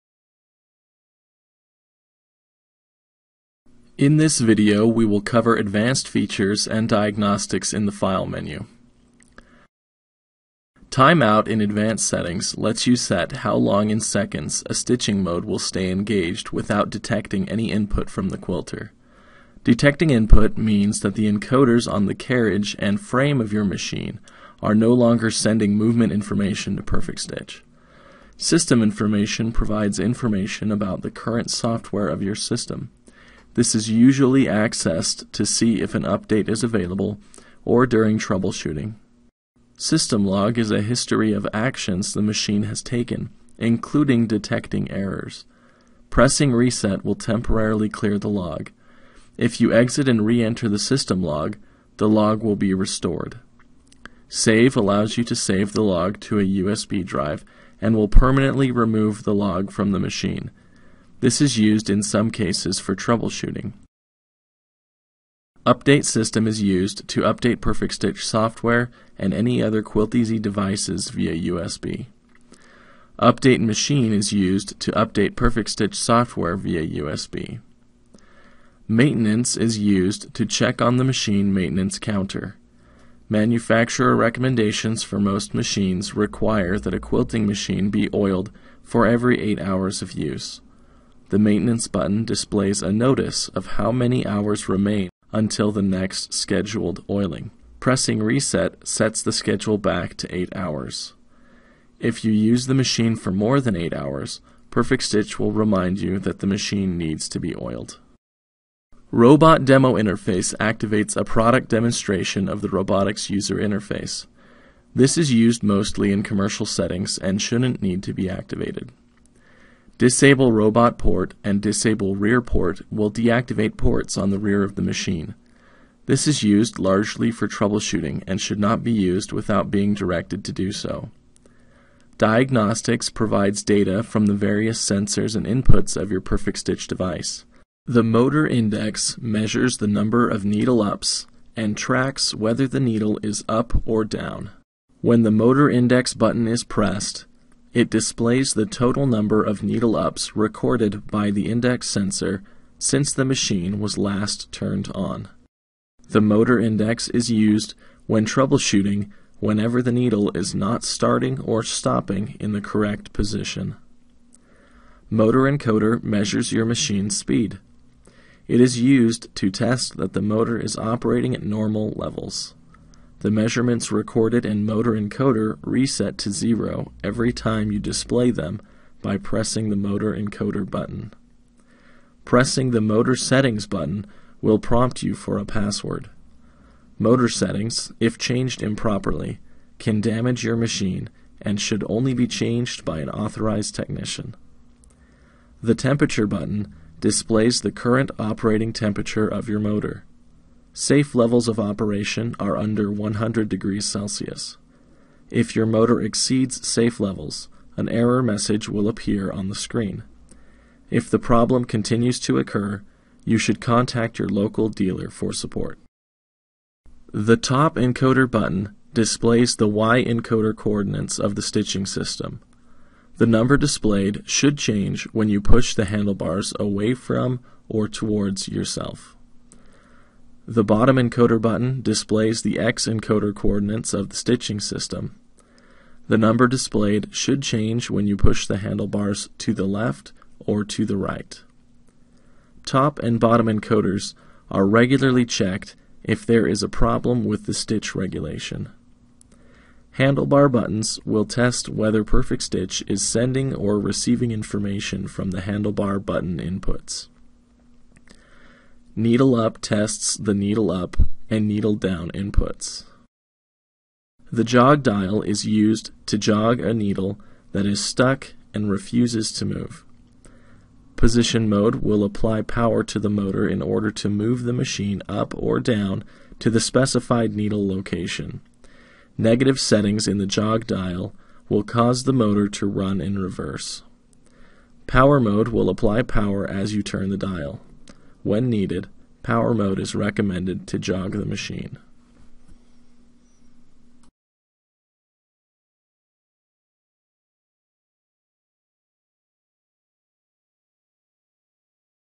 Perfect Stitch Tutorial 03 - Advanced and Diagnostics
An overview of the advanced and diagnostics tools in the Perfect Stitch File menu.